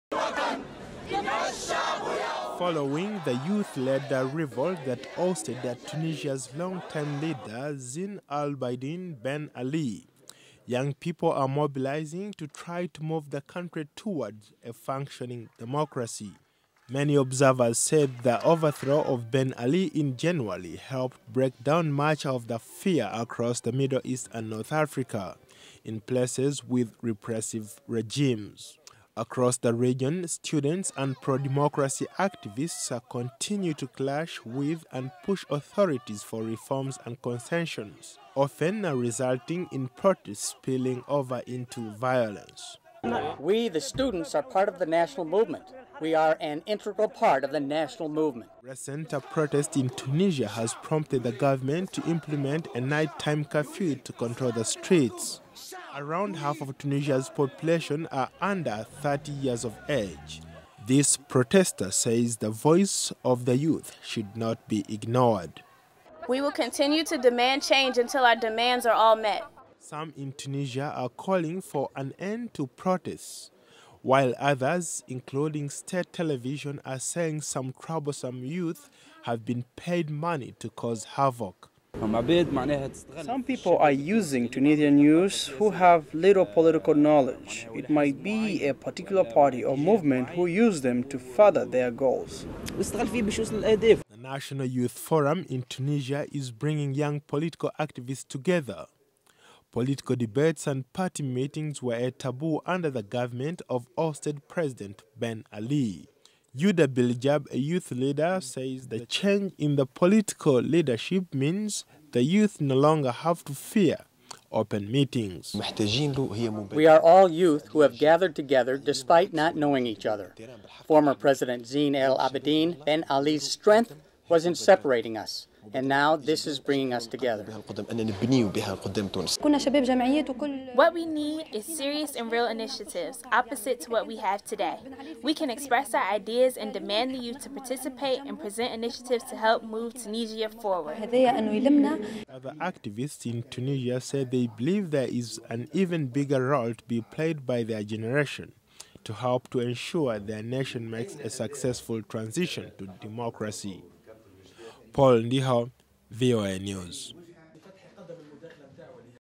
Tunisia Youth
Tunisian Youth are playing a active roles and have recently become more politicized as they continue to assert themselves, and demand for democratic change.